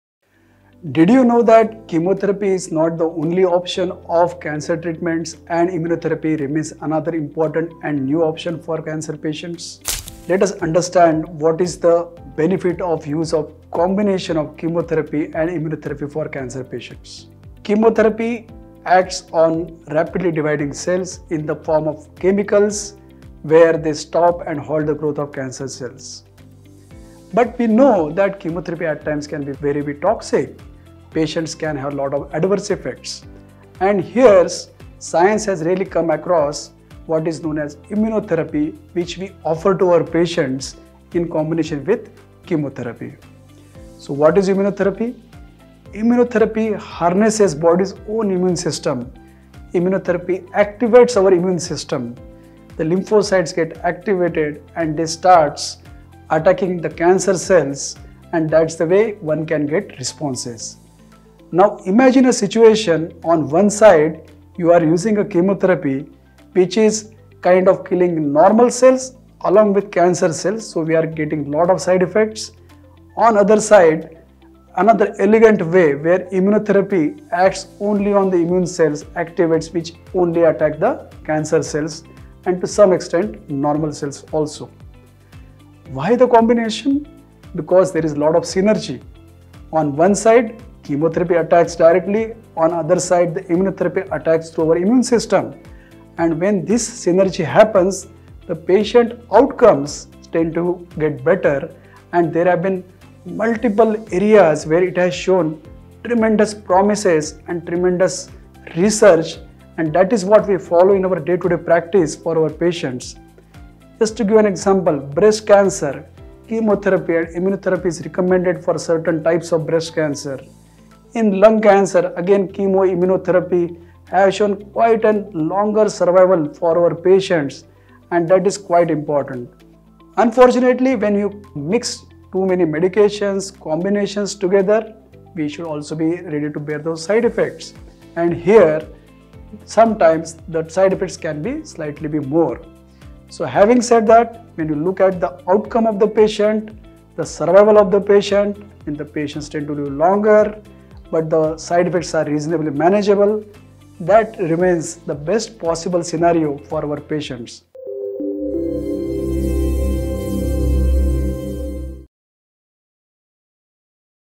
What if chemotherapy and immunotherapy worked together to fight cancer? | Dr Nikhil S Ghadyalpatil
Dr. Nikhil S Ghadyalpatil, Director-Medical Oncology at Apollo Hospitals, Jubilee Hills, Hyderabad, explains how combining chemotherapy with immunotherapy creates a powerful synergy, boosting survival rates and keeping side effects under control.

👉 For more information on the latest advancements in treatments and procedures, visit the Apollo Hospitals Hyderabad website at